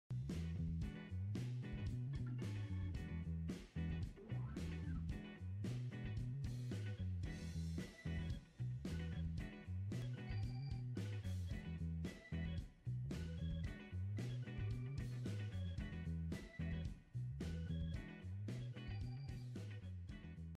Cocktails Dry Ice
A simple cocktail that tastes delicious and looks amazing with a dry ice fog cloud.